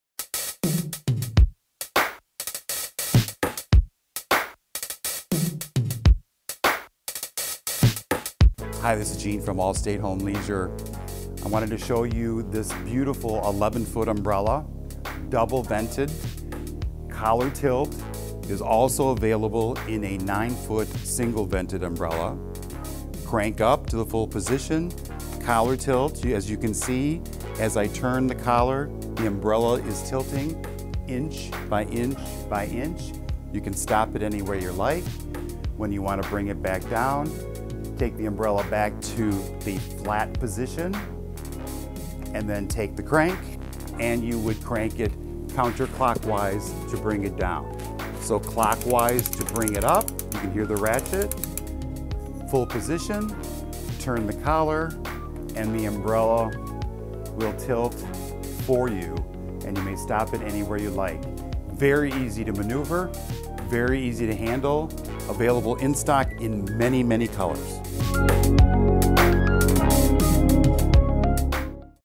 Allstate Patio Review Treasure Garden 11ft Umbrella
When you are looking for a superior quality patio market umbrella with time tested fabric you needn't go any further. Our 11-foot Treasure Garden Auto Tilt Umbrella is built to last and has some of the most advanced features found on any outdoor umbrella today! This umbrella features a durable powder coat aluminum (rust free) frame, topped with 100% durable Obravia2 solution dyed polyester fabric ensuring many years of beautiful carefree operation.